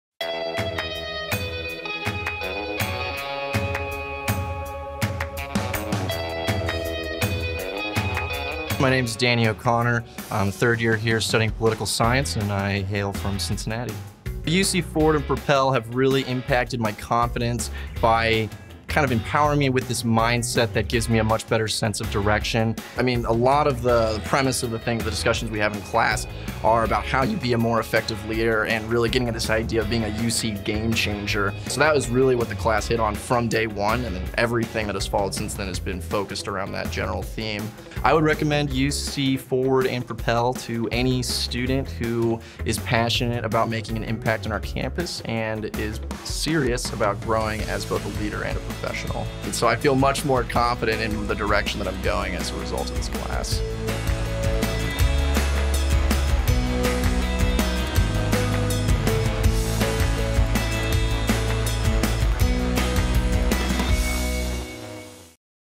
UC ProPEL - Danny's Story of Experiential Learning at the University of Cincinnati
Danny is a student from the University of Cincinnati, majoring in Political Science and Cyber Security.  This is Danny's story of building leadership skills through experiential learning in a transdisciplinary UC Forward course offered by UC ProPEL. Part of our "Transformation Through Experience" series of student success stories.